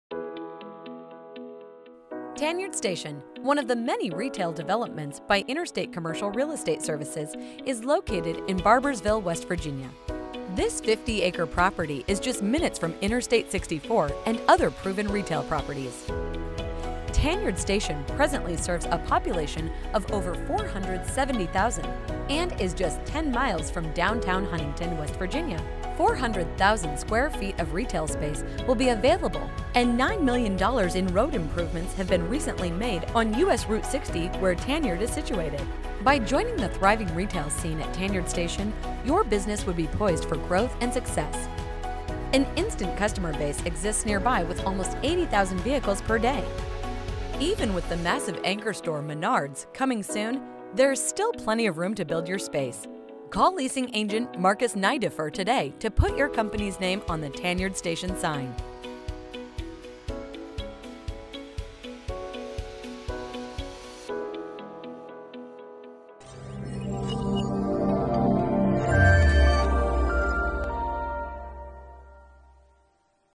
Tanyard Station
Tanyard Station, the region's newest retail development, is now accepting leases in Barboursville, WV.